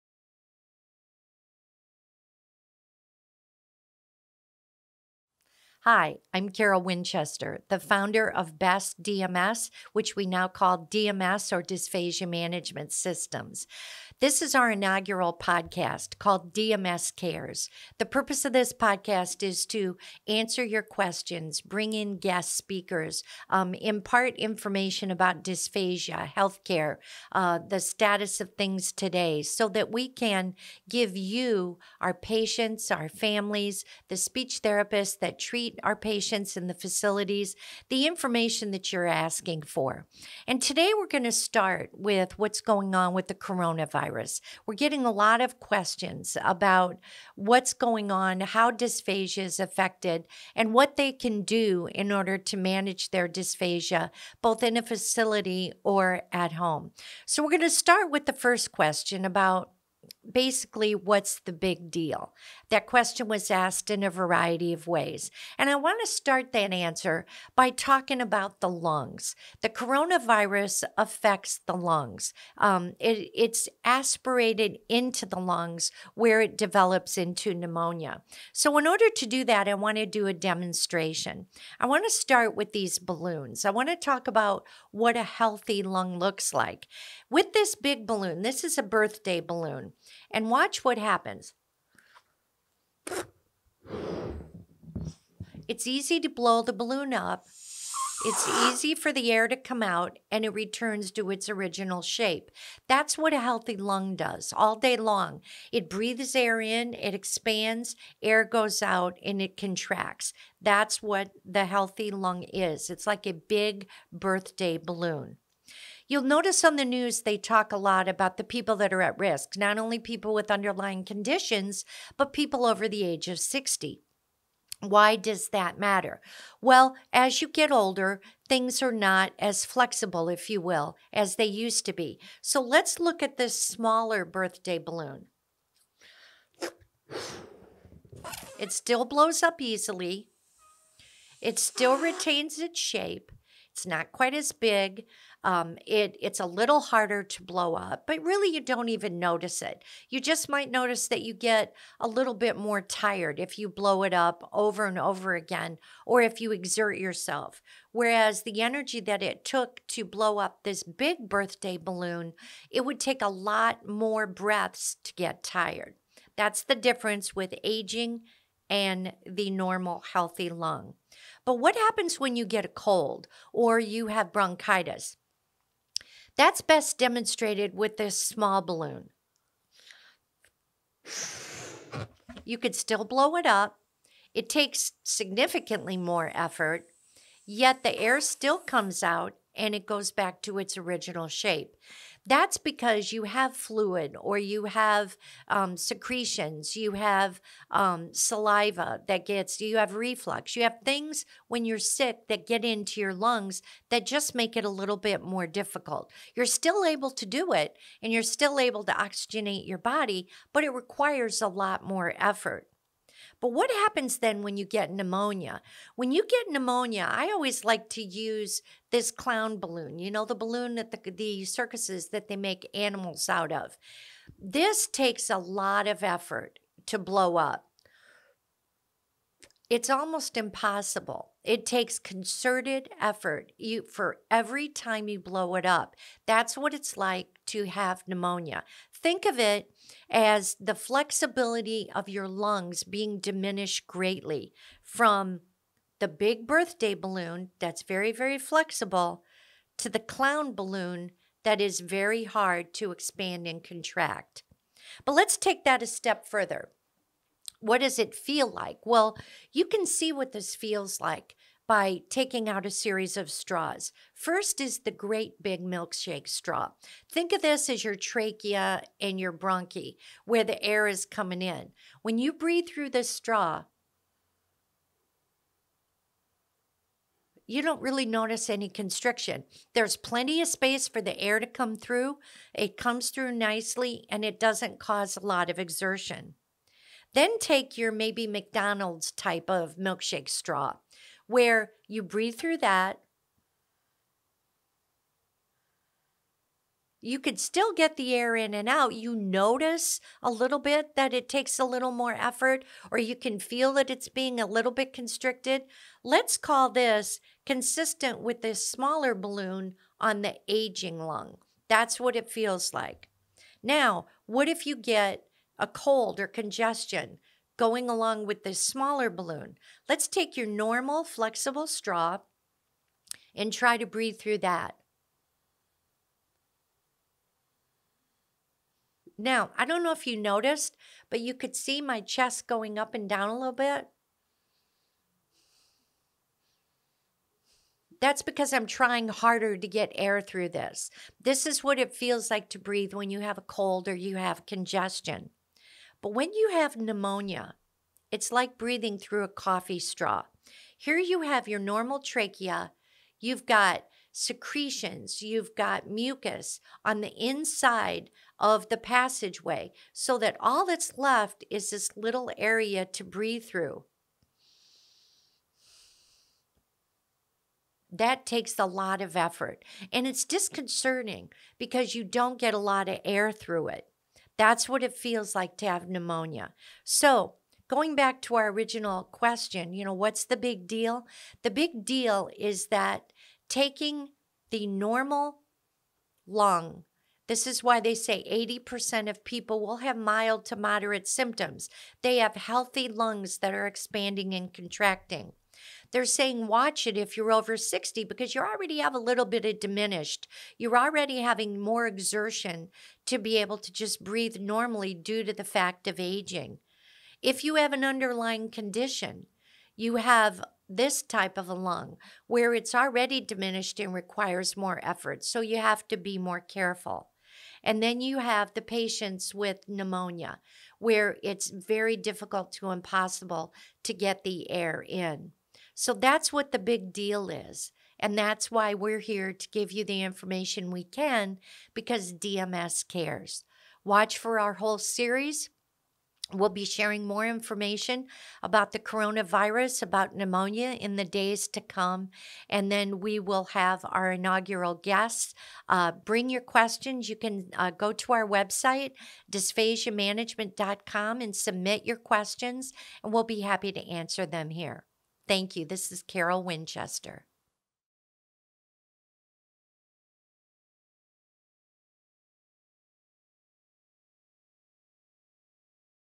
DMS Cares - Dysphagia, Pneumonia & The Coronavirus
Today’s episode answers the question: “What’s the big deal with the CoronaVirus and pneumonia? I don’t understand why I should be worried."

We will answer this question with a demonstration on how the lungs work and what it’s like to try to breathe with a respiratory complication.

Consult your speech pathologist about whether this education is right for you.
If you’re having trouble swallowing but haven’t been diagnosed with dysphagia, talk to your doctor.